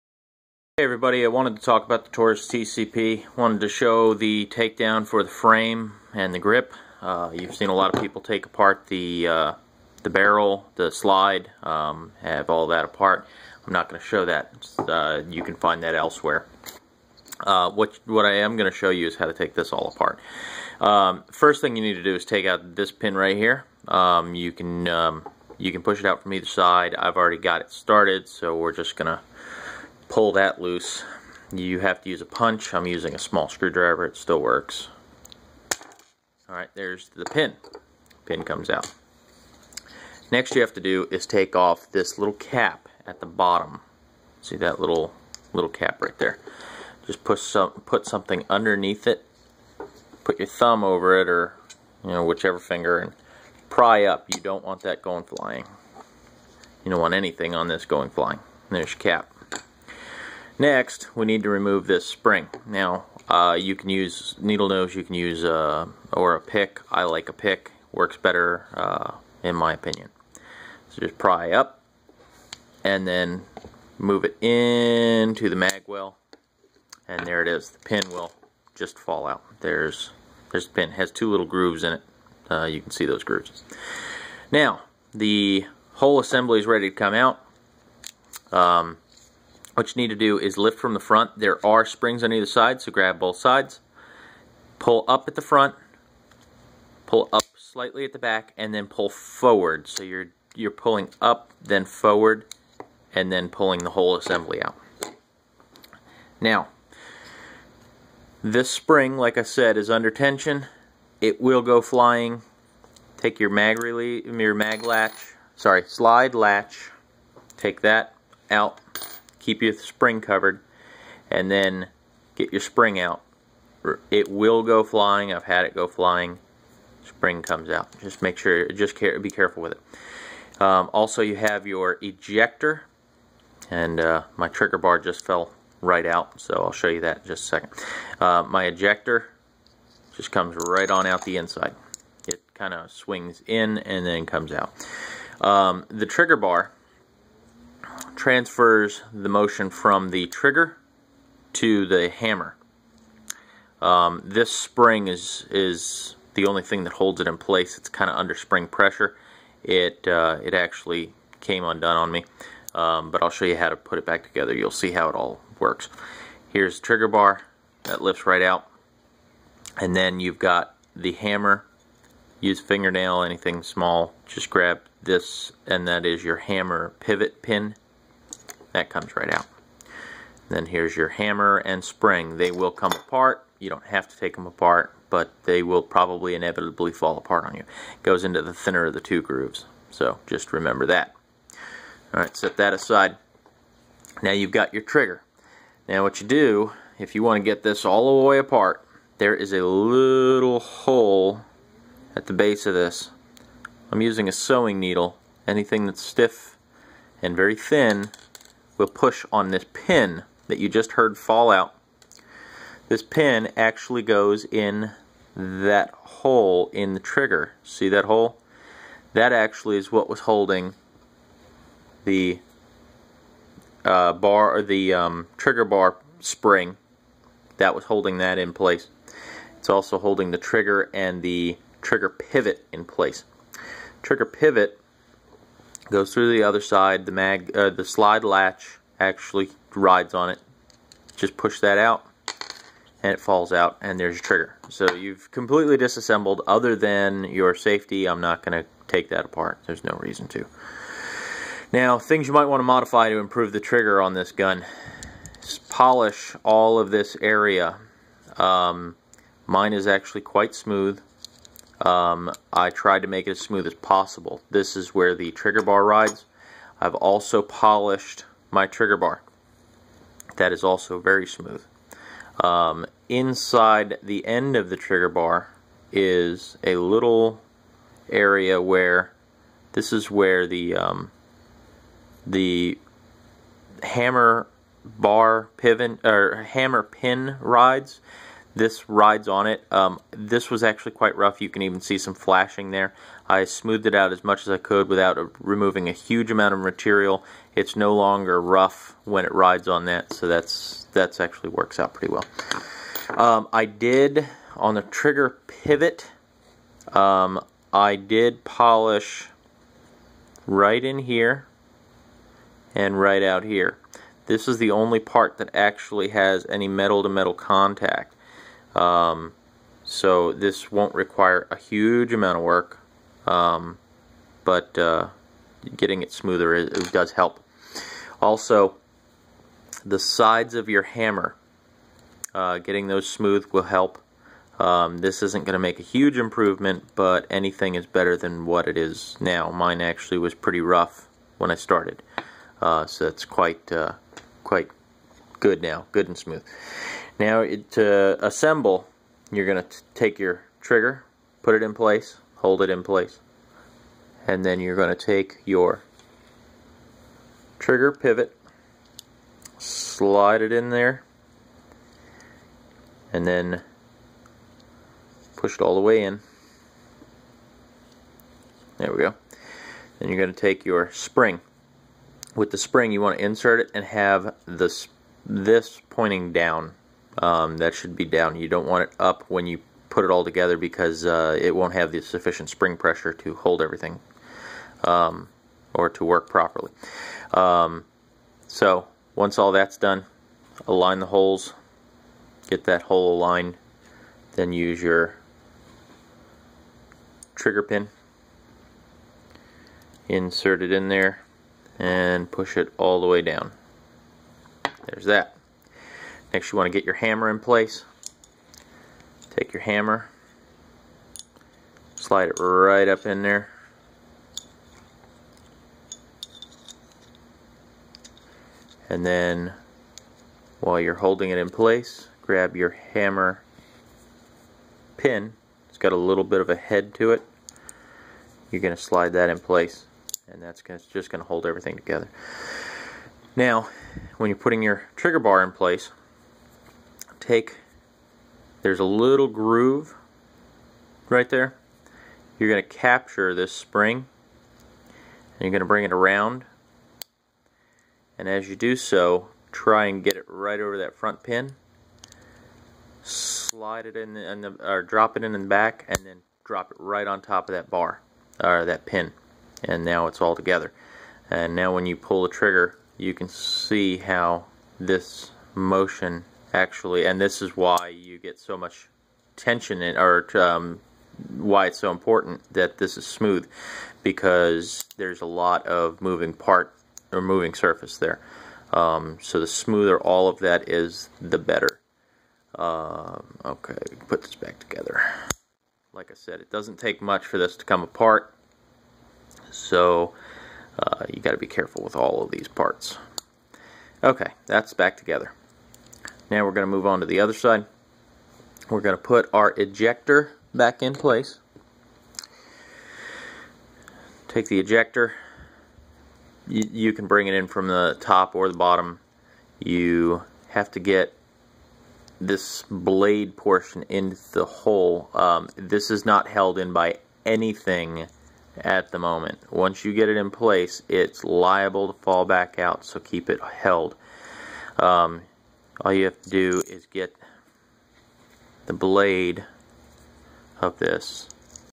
Taurus TCP Frame disassembly part 1
Dissasembly of Taurus TCP frame and grip.  With some discussion on improving trigger feel.